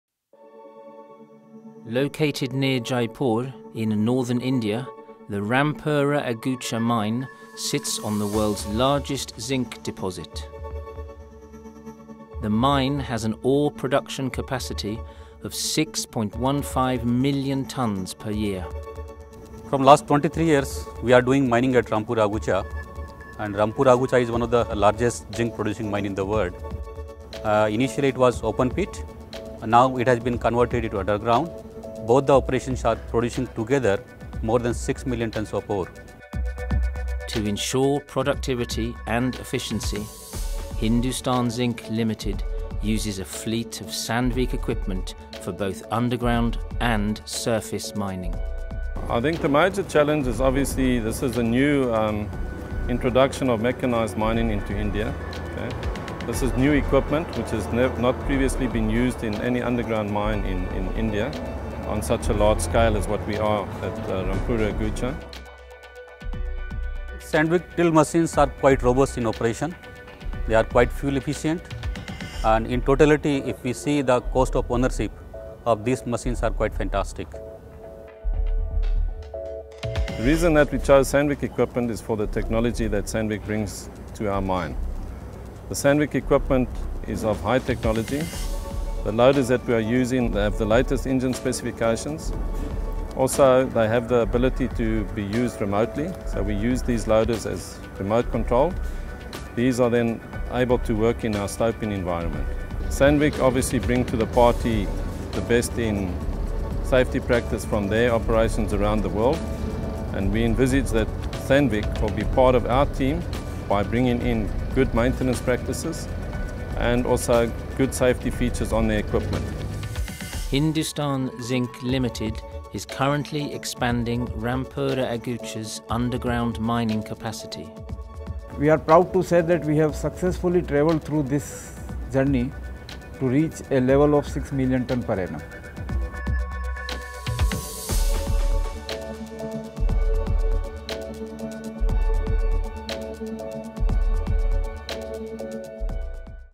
Zinc in sync
Located near Jaipur, in Northern India, the Rampura Agucha mine sits on the world's largest zinc deposit. The mine is expanding its ore production capacity of more than 6 million tonnes per year. To ensure productivity and efficiency, Hindustan Zinc Ltd relies on a fleet of Sandvik Mining equipment.